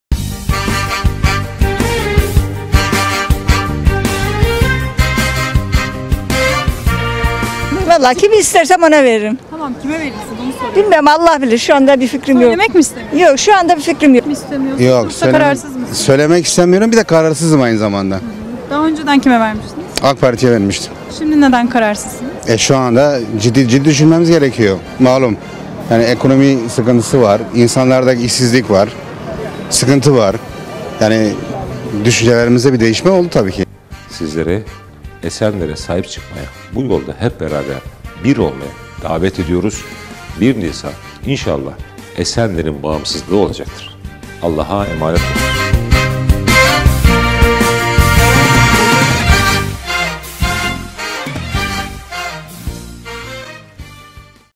Esenler'de Seçmen Olmak...
Unutmayın! Sizlerin iradesini hiçe sayıyorlar... Hayat bisiklet sürmek gibidir. Hedefinize ulaşabilmeniz için, Hareket etmeniz ve dengede kalmanız gerekir... Biz hiç bir partiniz alternatifi değiliz... ESENLER'in güzel insanlarına lütfet dilmeden, hak ettiği hizmet anlayışını İNŞALLAH Sizlerle getireceğiz...